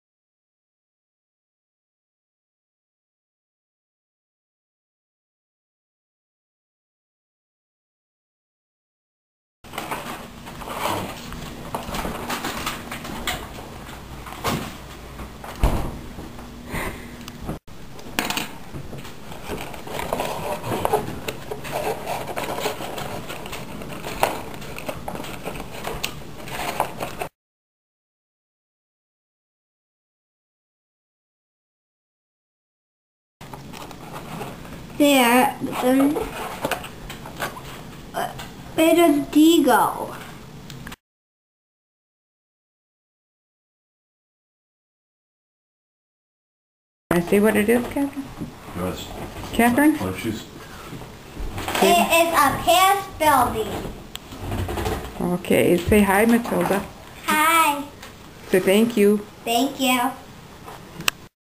A Street in Paris
Catherine puts together her paper buildings in Paris